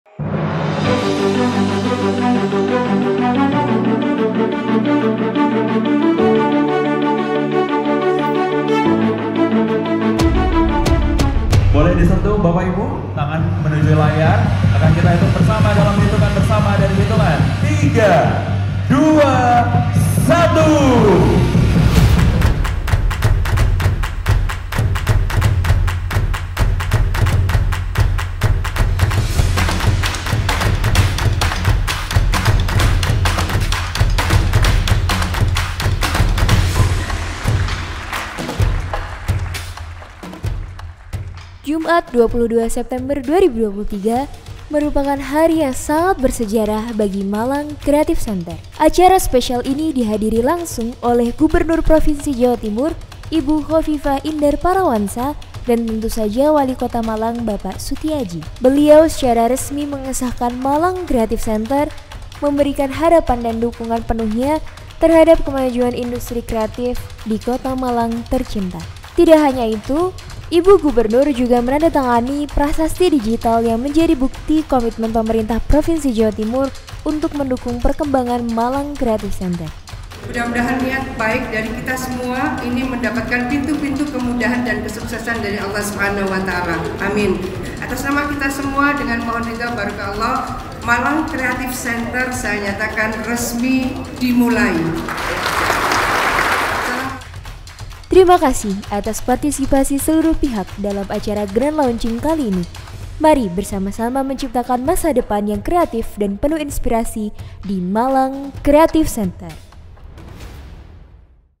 AFTER MOVIE GRAND LAUNCHING MCC
Jumat 22 September 2023 merupakan hari yang sangat bersejarah bagi Malang Creative Center, akhirnya Malang Creative Center diresmikan dalam acara Grand Launching yang spektakuler. Acara spesial ini dihadiri langsung oleh Gubernur Provinsi Jawa Timur, Ibu Khofifah Indar Parawansa dan tentu saja Wali Kota Malang, Bapak Sutiaji.

Beliau secara resmi mengesahkan Malang Creative Center dan memberikan dukungan penuhnya terhadap kemajuan industri kreatif di kota Malang tercinta.

Tidak hanya itu, Ibu Gubernur juga menandatangani prasasti digital yang menjadi bukti komitmen pemerintah provinsi Jawa Timur untuk mendukung perkembangan Malang Creative Center.

Terima kasih atas partisipasi seluruh pihak dalam acara Grand Launching kali ini. Mari bersama-sama menciptakan masa depan yang kreatif dan penuh inspirasi di Malang Creative Center.